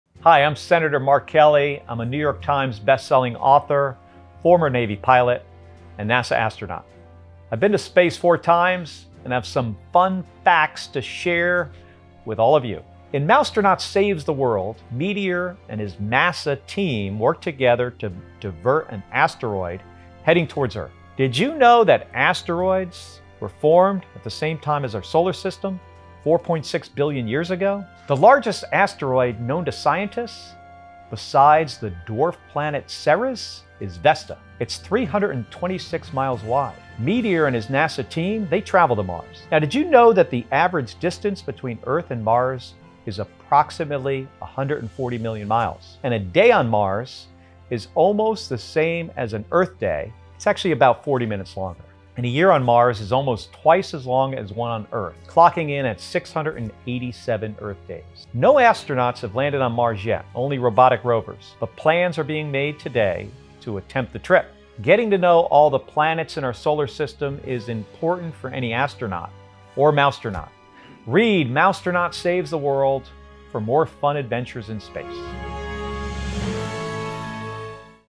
Senator Mark Kelly shares space fun facts!
Mark Kelly is the US Senator of Arizona, a New York Times bestselling author, former Navy pilot, and NASA astronaut.  These fun facts about space can be found in his new book, Mousetronaut Saves the World!, available now!